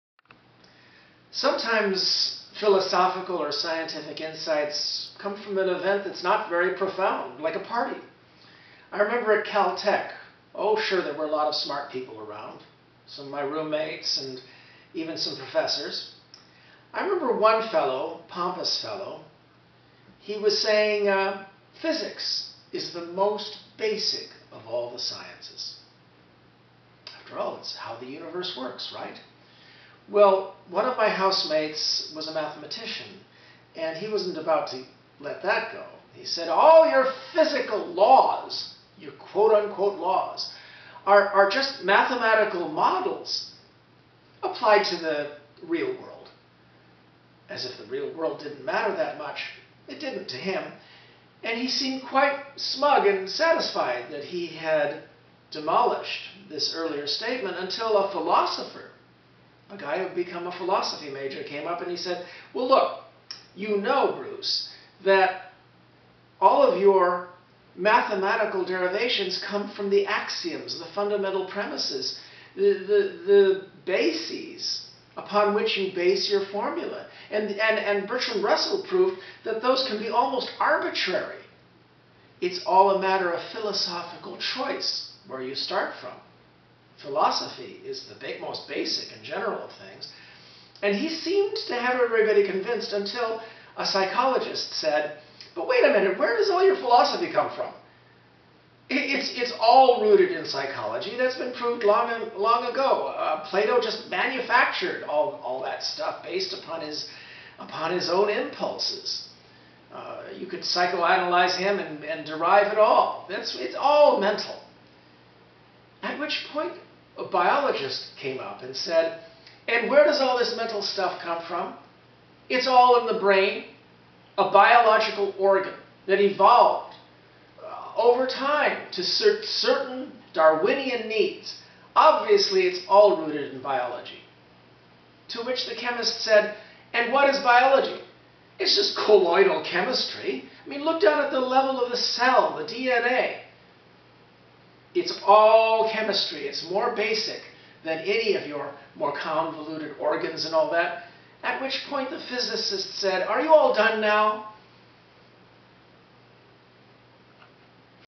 Which Science is the Most Basic?
Physics might be considered the most fundamental of all sciences, for all other sciences derive from basic principles of forces, motion, electromagnetism and thermodynamics. And yet, physical laws are mathematical models of the world; however, mathematics itself is abstract, deriving from theoretical constructs of philosophy. But, philosophy arises out of theories of mind, or psychology. The mind itself depends upon the biology of the brain. which is nothing but chemical reactions of molecules, such as neurotransmitters and proteins. And of course, chemistry depends upon the behavior of atoms and forces, which is constrained by physics. Physics, mathematics, philosophy, biology, chemistry, or physics -- the eternal loop.

David Brin's books include The Postman, Startide Rising,and The Uplift War, as well as his non-fiction work, The Transparent Society, dealing with issues of privacy and secrecy.